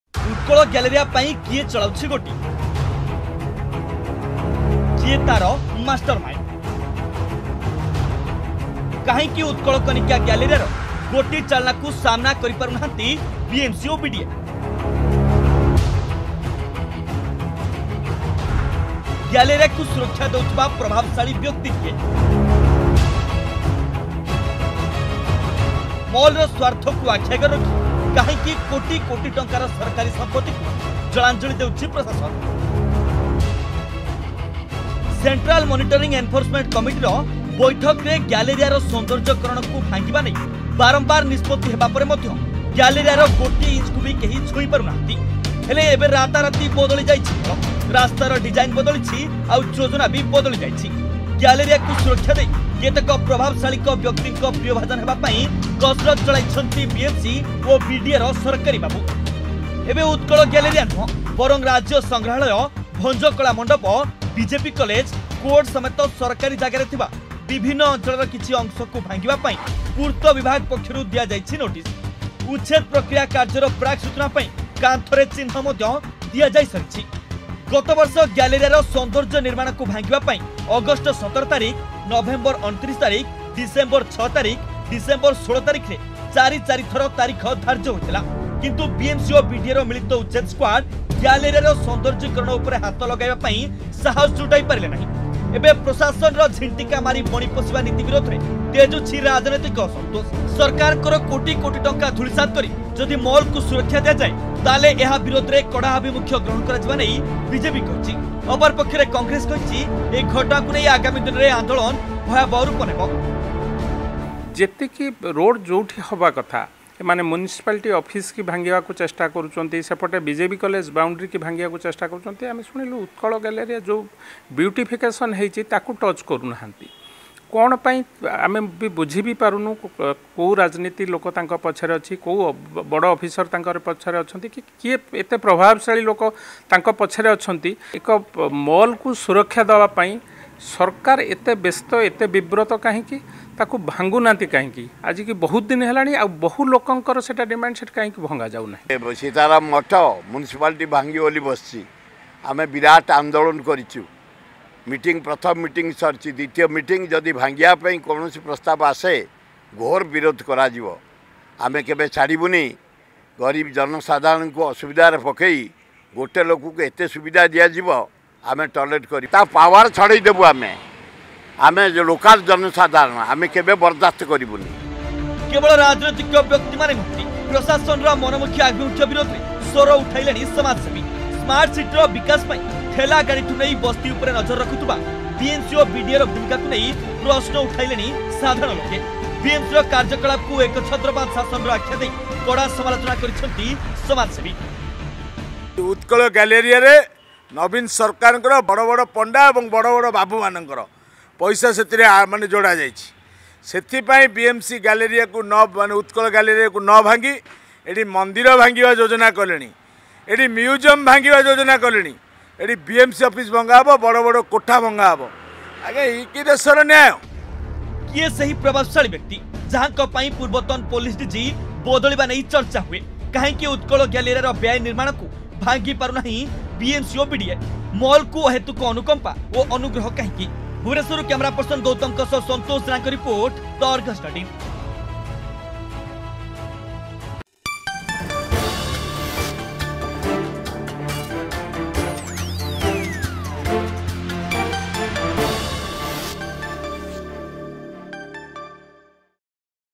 ଉତ୍କଳ ଗ୍ୟାଲେରିଆ ପାଇଁ କିଏ ଚଳାଉଛି ଗୋଟି ...
ସେଣ୍ଟ୍ରାଲ ମନିଟରିଂ ଏନଫୋର୍ସମେଣ୍ଟ ମନିଟରିଂ କମିଟିର ବୈଠକରେ ଗ୍ୟାଲେରିଆରର ସୈନ୍ଦର୍ଯ୍ୟକରଣକୁ ଭାଙ୍ଗିବା ନେଇ ବାରମ୍ବାର ନିଷ୍ପତ୍ତି ହେବା ପରେ ମଧ୍ୟ ଗ୍ୟାଲେରିଆର ଗୋଟିଏ ଇଞ୍ଚକୁ ବି କେହି ଛୁଇଁ ପାରୁନାହାନ୍ତି । ହେଲେ ଏବେ ରାତାରାତି ବଦଳିଛି ଖେଳ । ରାସ୍ତାର ଡିଜାଇନ ବଦଳିଛି ଆଉ ଯୋଜନା ବି ବଦଳିଛି ...